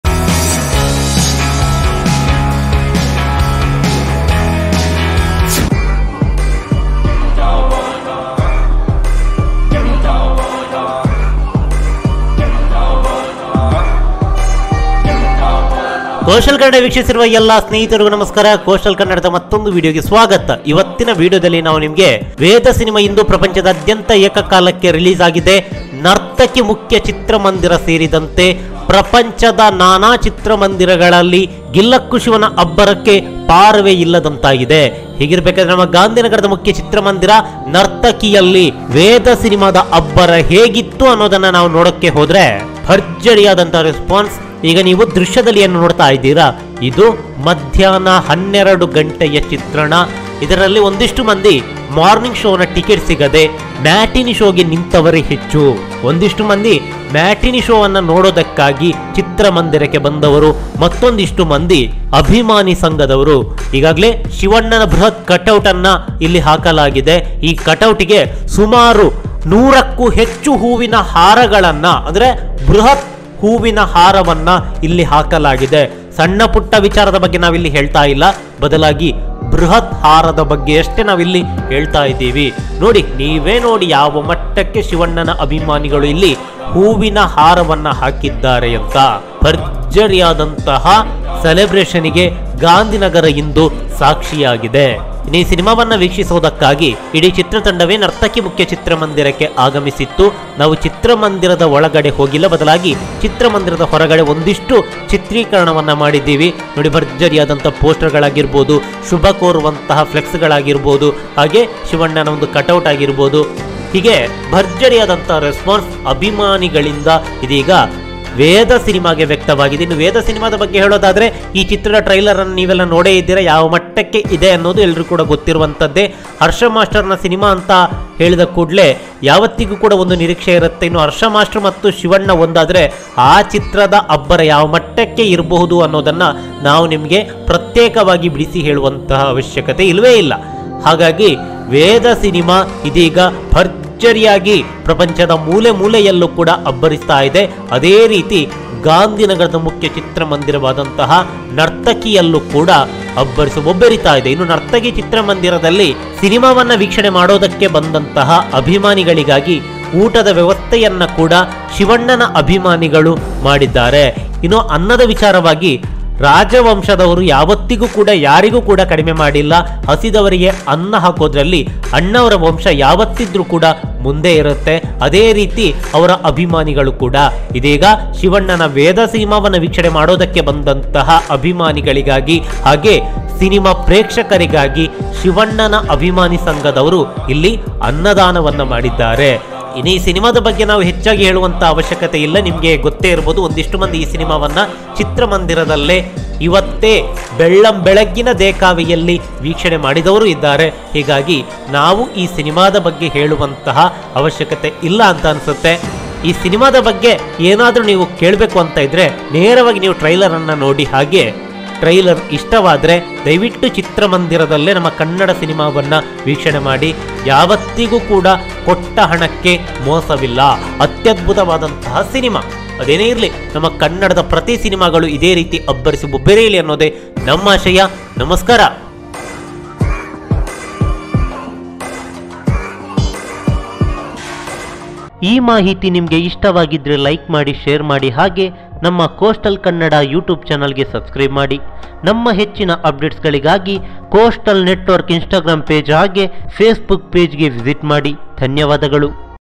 Veda Review : ನರ್ತಕಿಯಲ್ಲಿ ವೇದ ಅಬ್ಬರ.! ಅನ್ನದಾನ.. ಕಟೌಟ್.. ಟಿಕೇಟ್ ಸೋಲ್ಡೌಟ್.! ಸಂಭ್ರಮಕ್ಕೆ ಚಿತ್ರತಂಡ ಸಾಕ್ಷಿ
ನರ್ತಕಿ ಚಿತ್ರಮಂದಿರ ಸೇರಿದಂತೆ ಪ್ರಪಂಚದಾದ್ಯ0ತ ಏಕಕಾಲಕ್ಕೆ ತೆರೆಕಂಡ ವೇದ ಚಿತ್ರಕ್ಕೆ ಭರ್ಜರಿಯಾದ ರೆಸ್ಪಾನ್ಸ್ ವ್ಯಕ್ತವಾಗ್ತಿದೆ. ಮುಖ್ಯ ಚಿತ್ರಮಂದಿರದಲ್ಲಿ ಜಾತ್ರಾ ಸಂಭ್ರಮ ಮನೆ ಮಾಡಿದೆ. ಅಭಿಮಾನಿಗಳು ಶಿವಣ್ಣನ ಬೃಹತ್ ಕಟೌಟ್‌ಗೆ ಹೂವಿನ ಹಾರ, ಪ್ಲೆಕ್ಸ್, ಲೈಟಿಂಗ್ ಹಾಕುವ ಮೂಲಕ ಸಂಭ್ರಮಿಸಿದ್ರು. ಅಷ್ಟೇ ಅಲ್ದೇ ಅನ್ನದಾನವನ್ನೂ ಮಾಡಿದ್ರು. ಇನ್ನೂ ಇಡೀ ಚಿತ್ರತಂಡವೇ ನರ್ತಕಿಯಲ್ಲಿ ಫಸ್ಟ್ ಡೇ, ಫಸ್ಟ್ ಶೋವನ್ನು ಅಭಿಮಾನಿಗಳ ಜೊತೆಗೆ ವೀಕ್ಷಿಸಿದ್ರು.